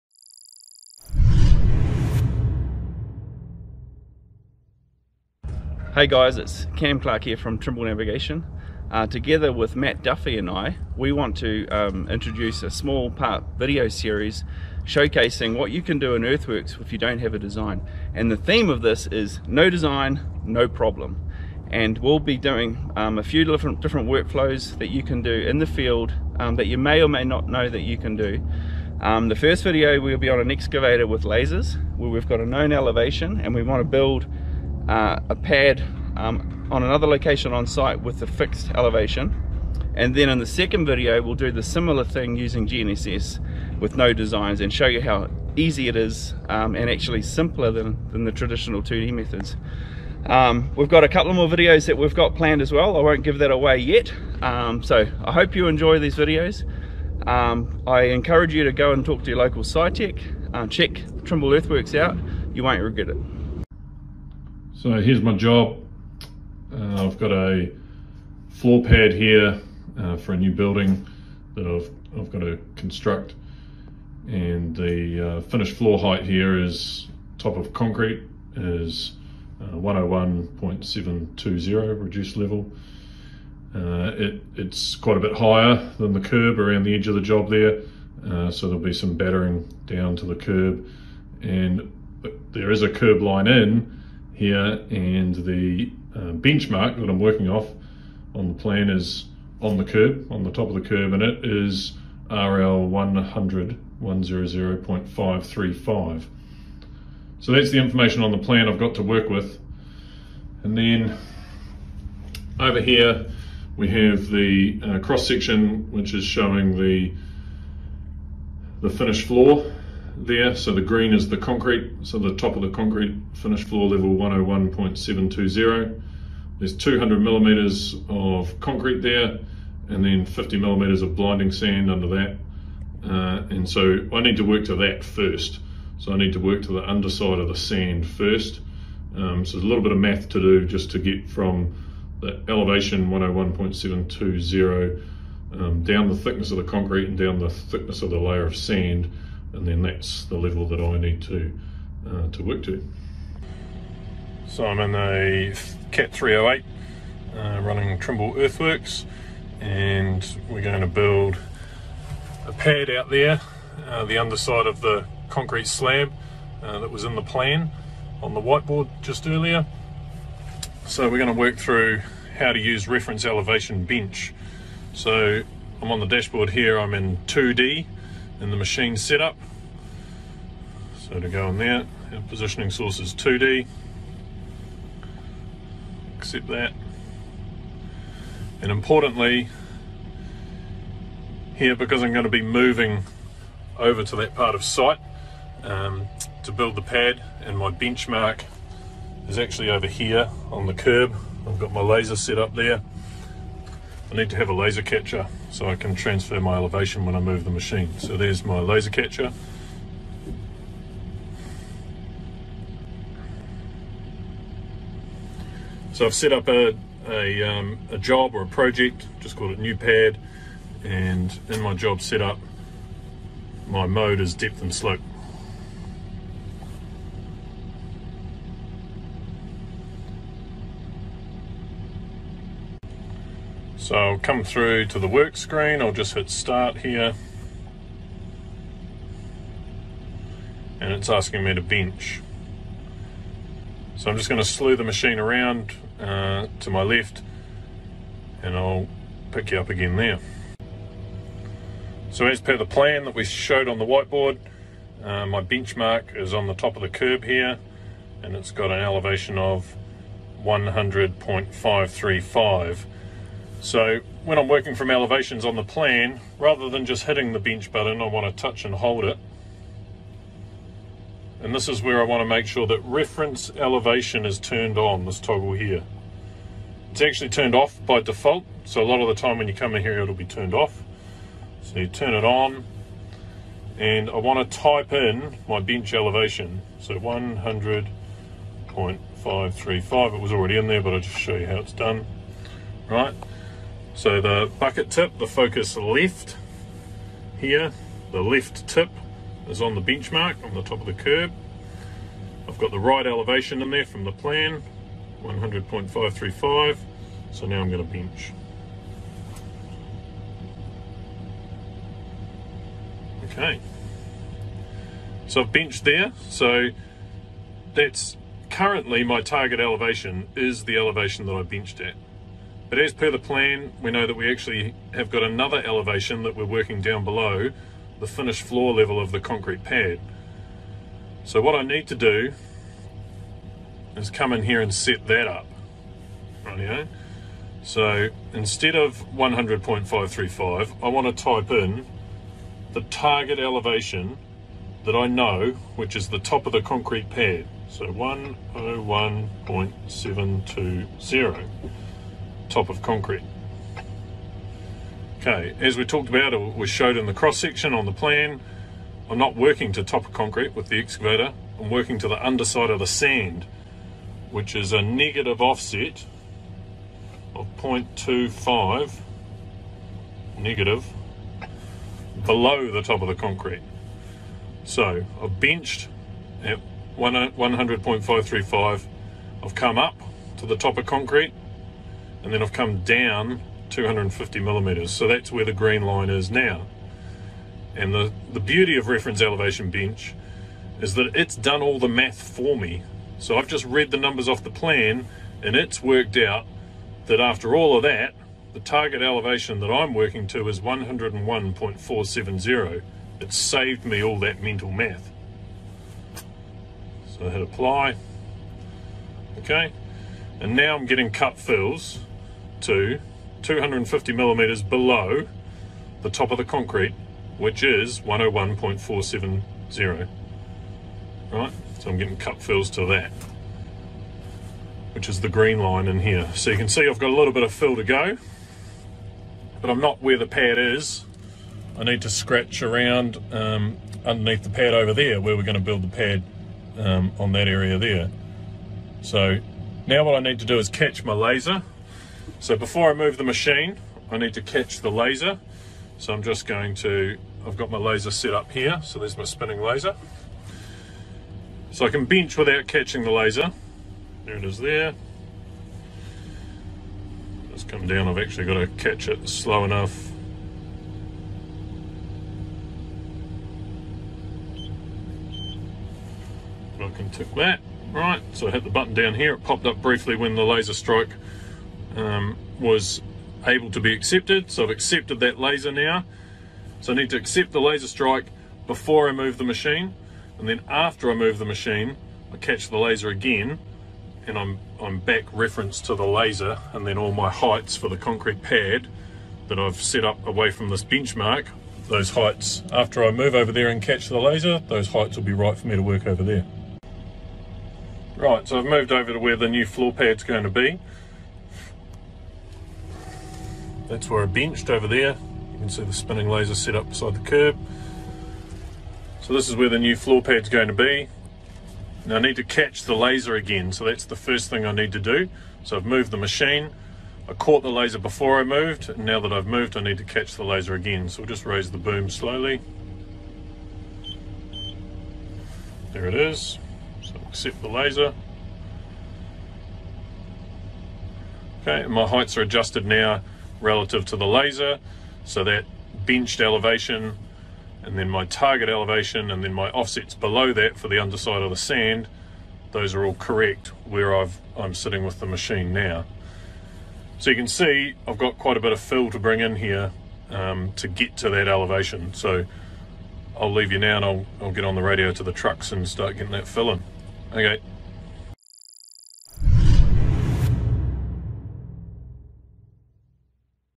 Trimble Earthworks: No Design, No Problem - Part 1
Two workflows that can be performed with Trimble Earthworks when you do not have a design.